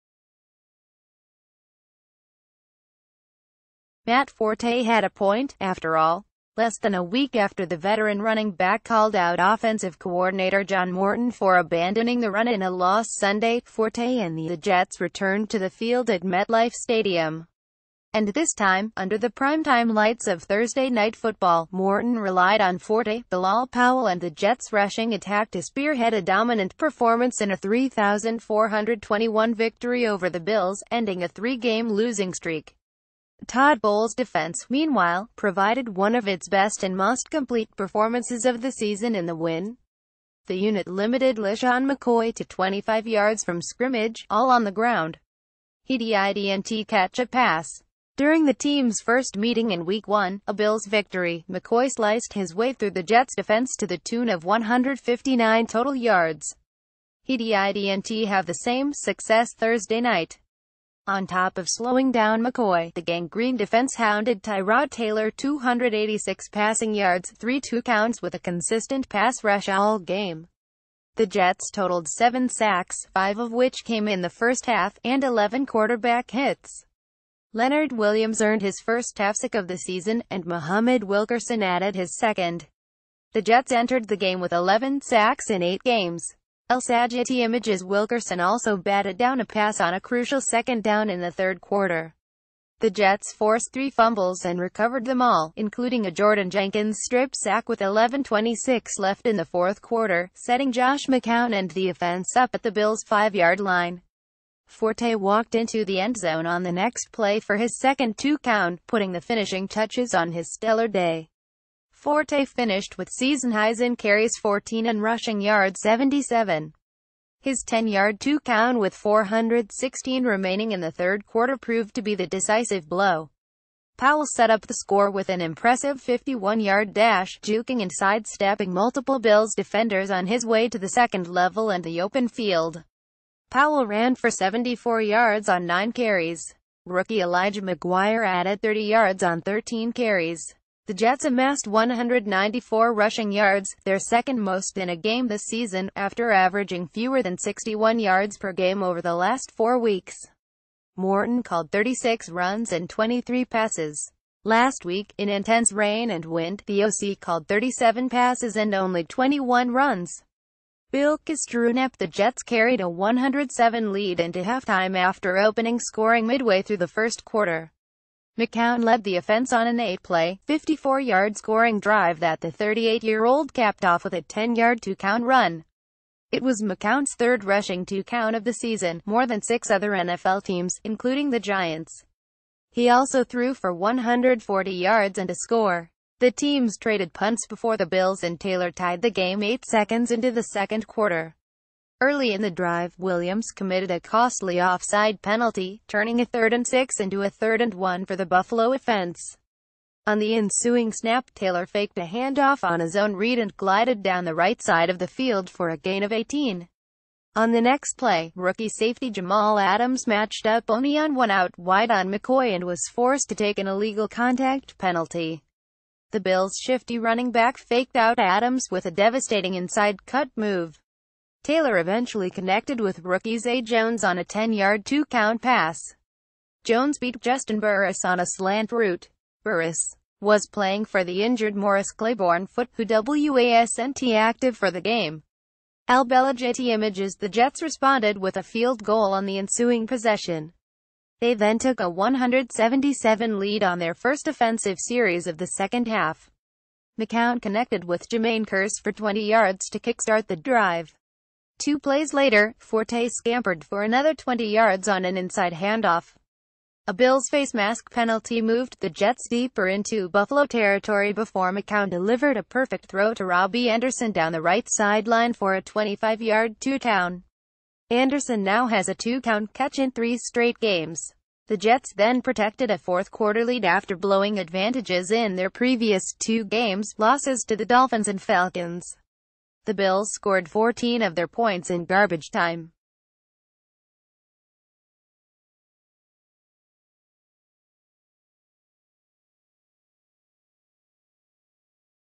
Jets dominate bills, 34-21, behind impressive run game, hounding defense| US Daily News
Jets dominate bills, 34-21, behind impressive run game, hounding defense
Matt Forte had a point, after all. Less than a week after the veteran running back called out offensive coordinator John Morton for abandoning the run...